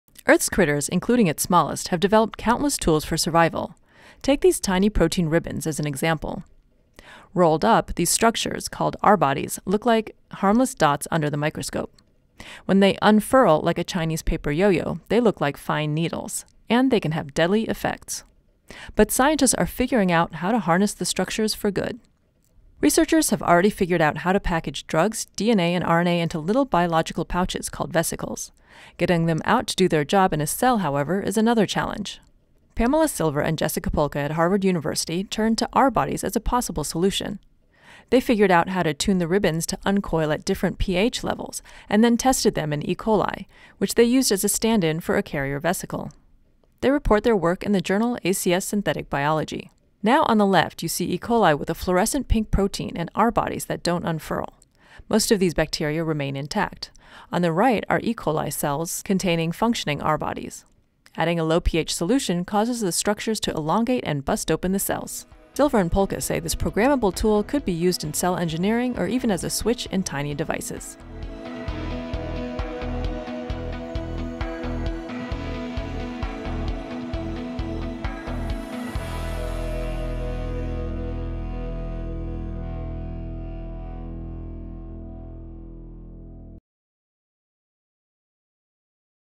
Punchy proteins could help advance drug delivery, MEMS devices
The fight to survive, even among single-celled organisms, can be nasty. Some use protein ribbons that are designed to puncture through membranes and release a deadly payload. Now scientists are harnessing them for good.